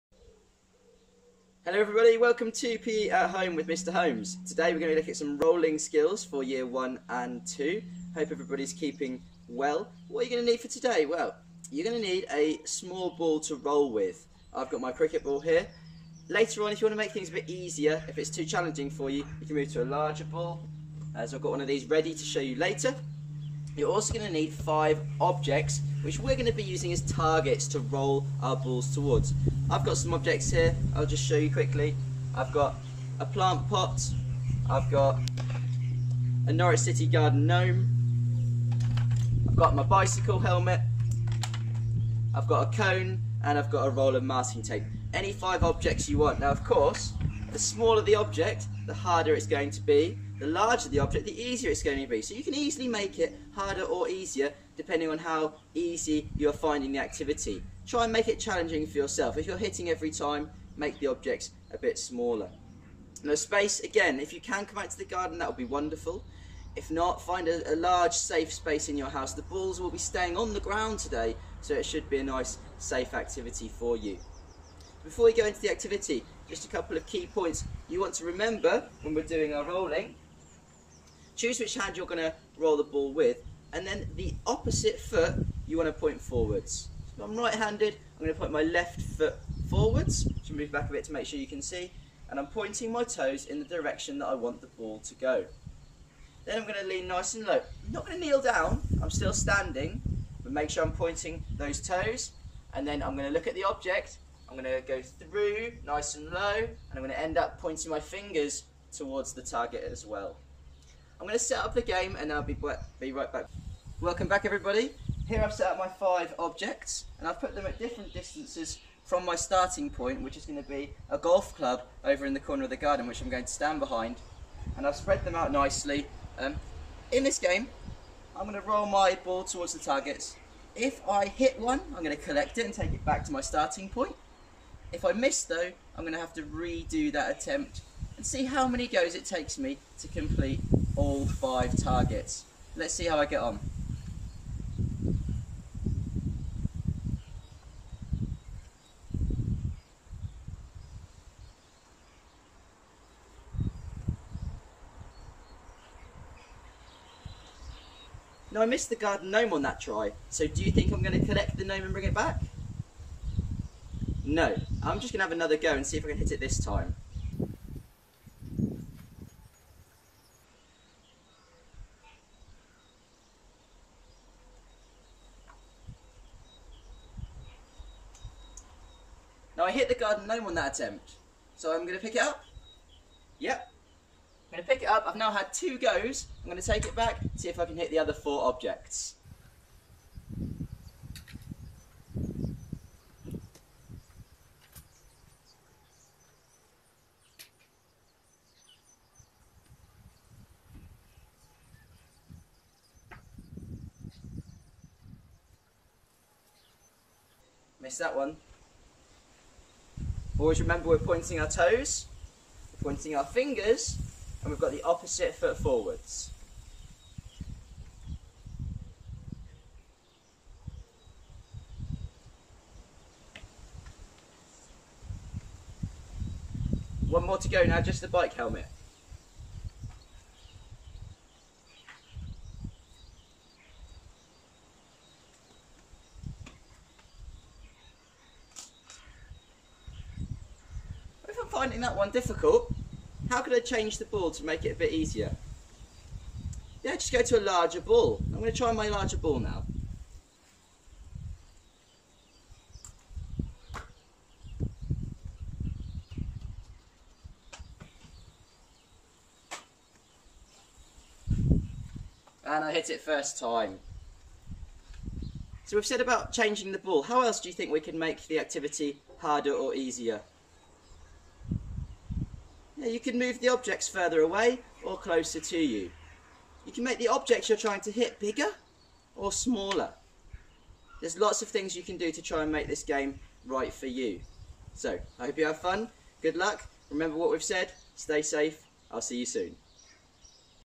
Rolling for accuracy - Year 1 and 2
Improve your rolling for accuracy with this fun target game!